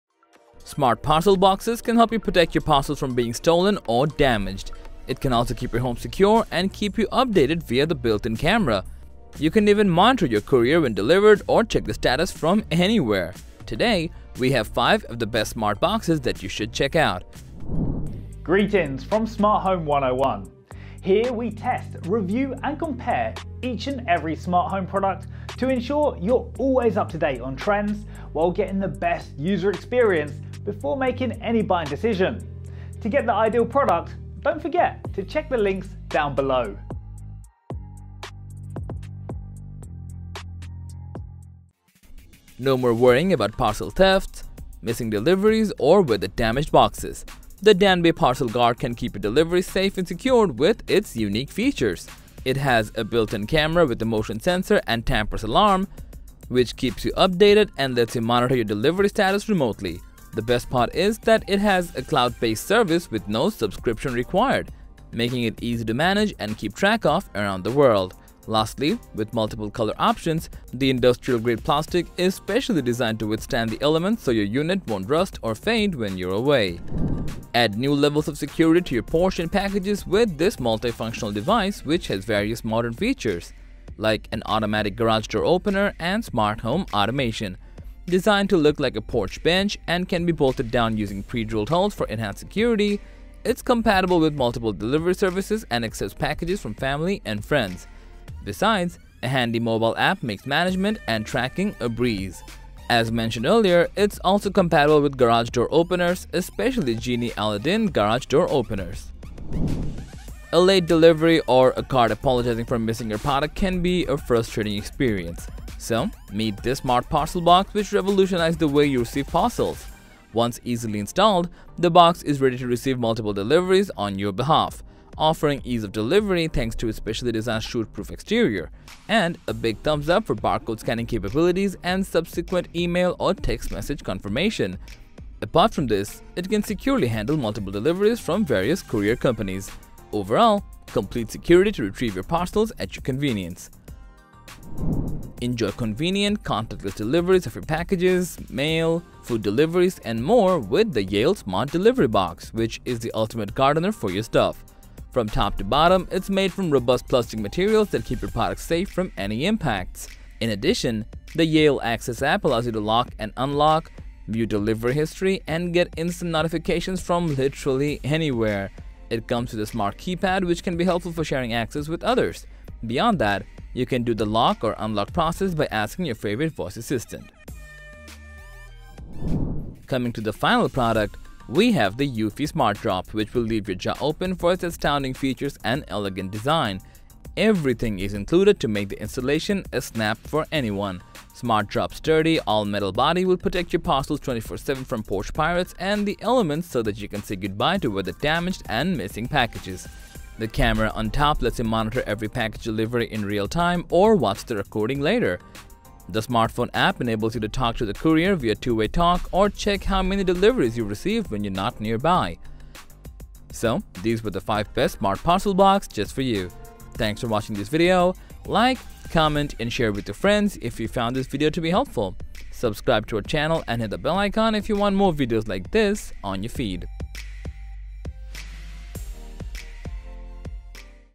5 Smart Parcel Box To Secure Your Deliveries!
With a smart parcel box, waiting for a package to arrive is a thing of the past, and it can save deliveries from any element. So, here is the list of the 5 best smart parcel boxes for convenient online deliveries.

Smart Parcel Box To Secure Your Deliveries Link: (Paid Links)